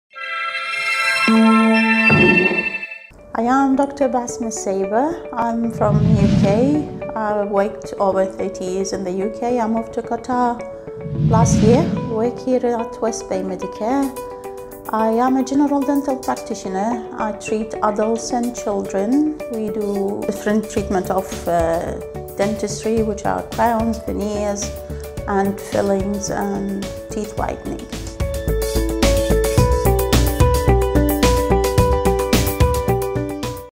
Dr. Basma provides a lot of treatments & services
د. بسمة صابر
طبيبة أسنان - ماجستير من المملكة المتحدة
مع خبرة ٢٩ عام
تجري الدكتورة بسمة المعالجات التالية: • طب الاسنان االعام و الأطفال • طب الأسنان الوقائي • علاجات الأسنان ٠تبيض الأسنان
اتصل الآن واحجز موعدك ٤٠٢٠٦٣٣٦

Meet Dr. Basma Saber
Master’s in Prosthetic Dentistry,
Member of the British Dental Association,
Member of the Faculty of General Dental Practitioners,
With more than 29 years experience.

Dr. Basma provides a lot of treatments & services: • General Dental practitioner • Prevention Dentistry including Children• Teeth Whitening* Restorative treatments